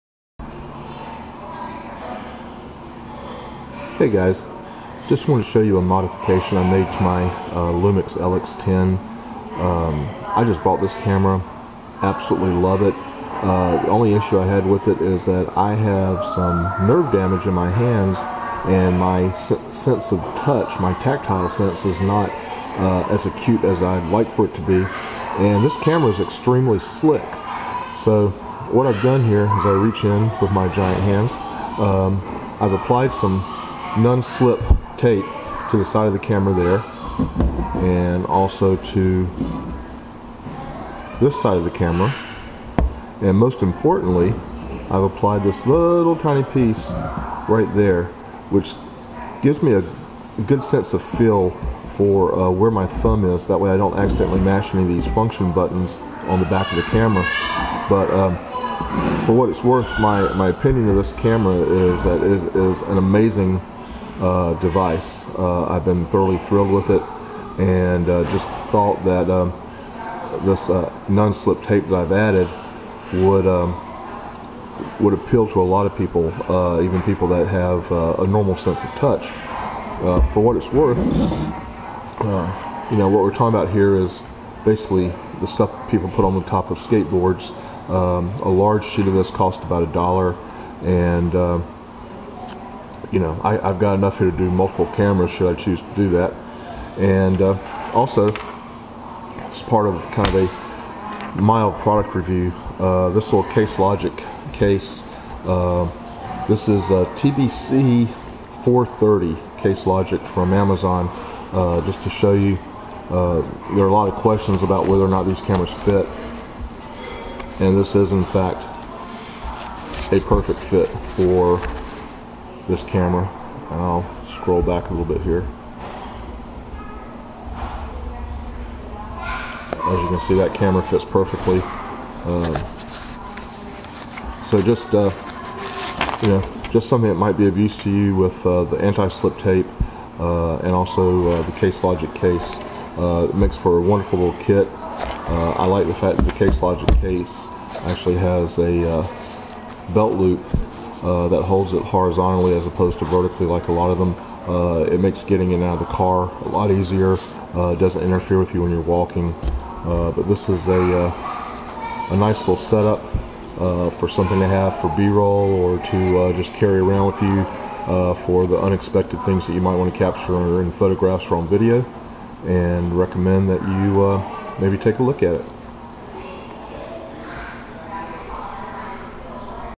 Panasonic Lumix LX10 Hack
Just a short vid showing a simple hack I have found to work really well. Also, a very brief product review of the Case Logic TBC-403 medium case. I misspoke in the video and said the model number was 430. It is the TBC-403, and was ~$9.00 on Amazon. As shown in the vid it is holding two spare batteries for the camera, and still fits the LX10 perfectly.

Shot in a coffee shop on a Panasonic Lumix FZ2500 so sorry for the background noise.